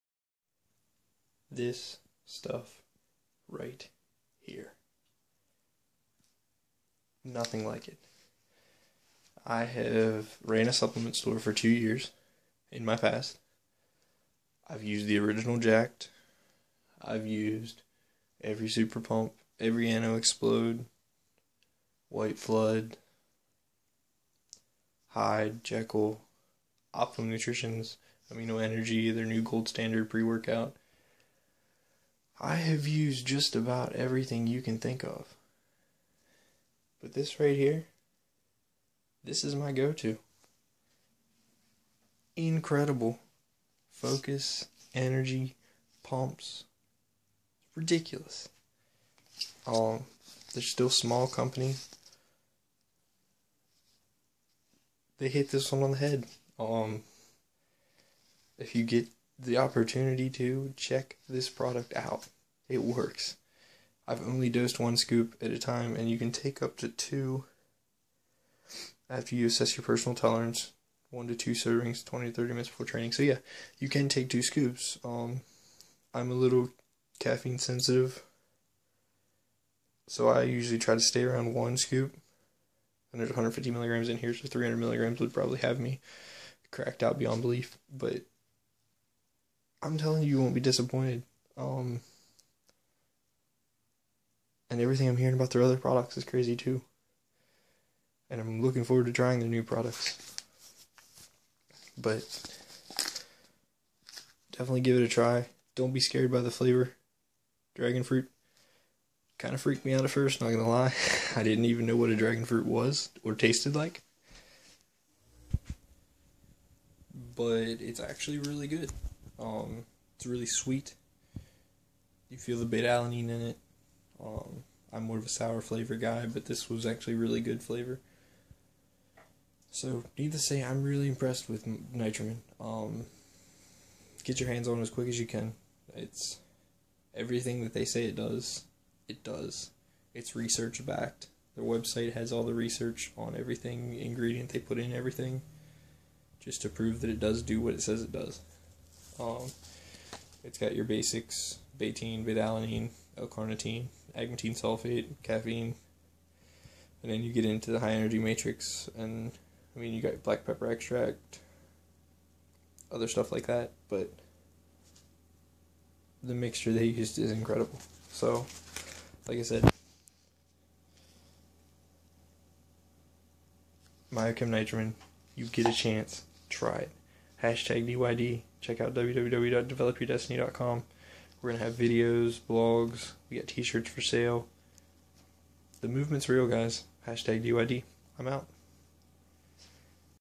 Myokem Nitramine Review
I give a quick break down review of the new pre workout myokem nitramine.